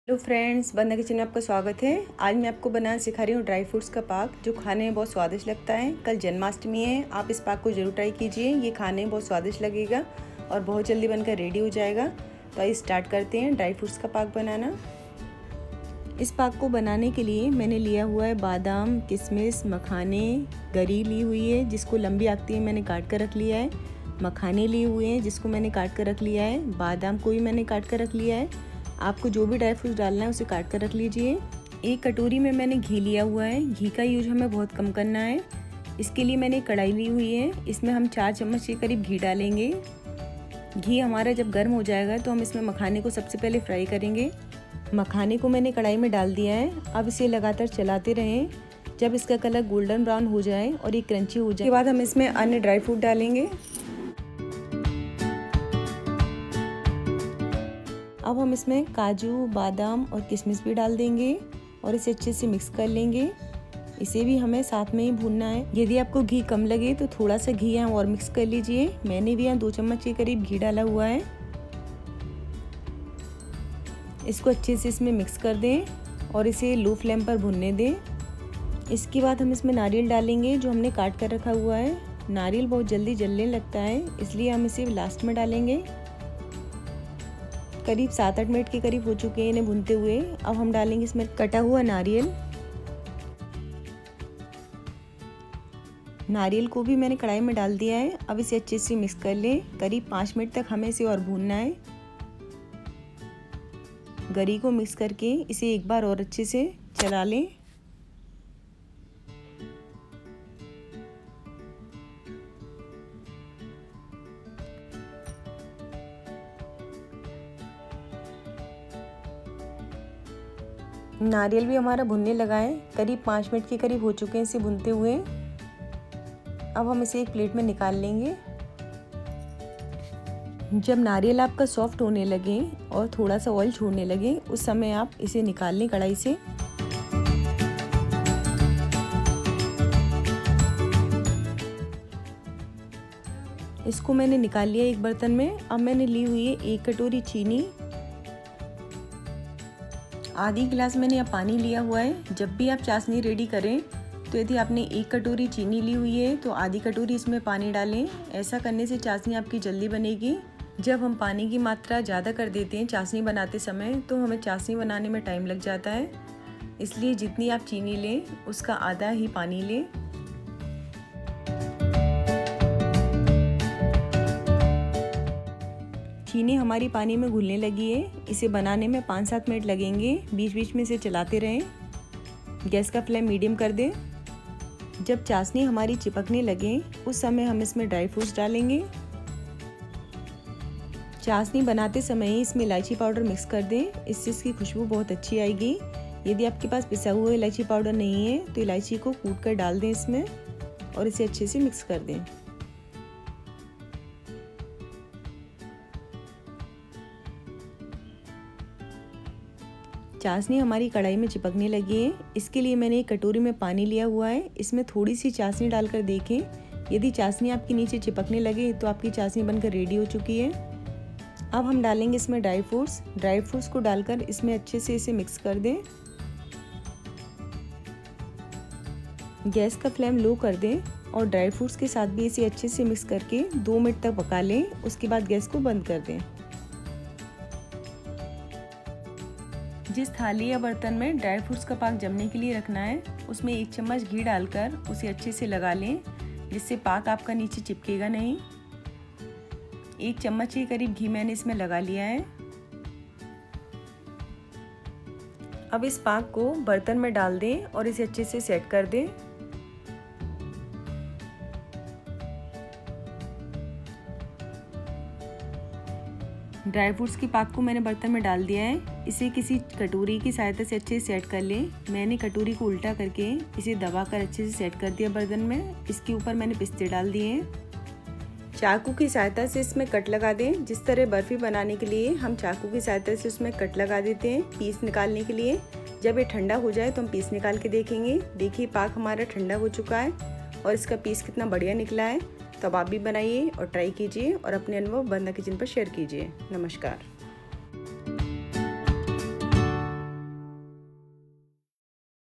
इस जन्माष्टमी पर मेवा पाक से लगाएँ कान्हाजी को भोग | Janmashtmi special Dry-fruit sweet
भगवान श्री कृष्णा को लगाये मेवा पाक का भोग।